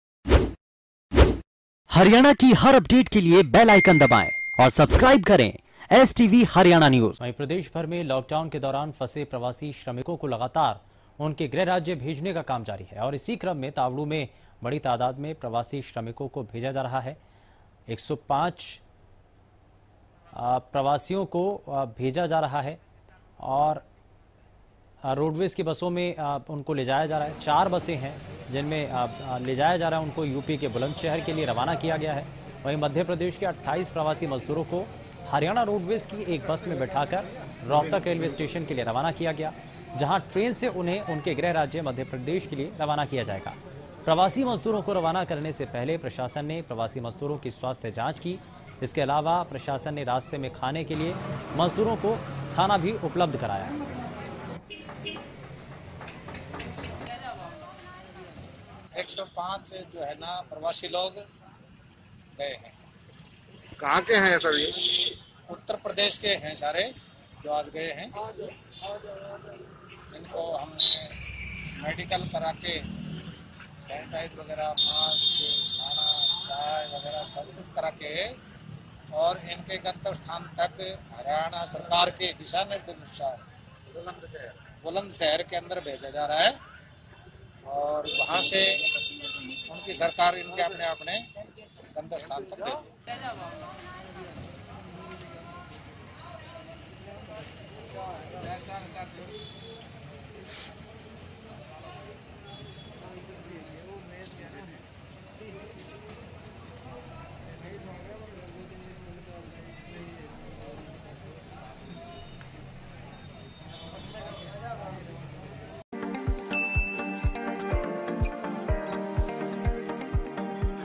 सोहना: श्रमिकों को गृह राज्य भेजने का काम जारी, 105 प्रवासी मजदूरों को बुलंदशहर के लिए किया रवाना
प्रदेश भर में लॉकडाउन के दौरान फंसे प्रवासी श्रमिकों को लगातार उनके गृह राज्य भेजने का काम जारी है इसी क्रम में तावड़ू में बड़ी तादाद में फंसे प्रवासी श्रमिकों को भेजा जा रहा है आज 105 प्रवासियों को हरियाणा रोडवेज के 4 बसों में भरकर यूपी के बुलंदशहर के लिए रवाना किया गया वही मध्य प्रदेश के 28 प्रवासी मजदूरों को हरियाणा रोडवेज की एक बस में बैठा कर रोहतक रेलवे स्टेशन के लिए रवाना किया गया जहां ट्रेन से उन्हें उनके गृह राज्य मध्य प्रदेश के लिए रवाना किया जाएगा…प्रवासी मजदूरों को रवाना करने से पहले प्रशासन ने प्रवासी मजदूरों की स्वास्थ्य जांच की गई...इसके अलावा प्रशासन ने रास्ते में खाने के लिए मजदूरों को खाना उपलब्ध कराया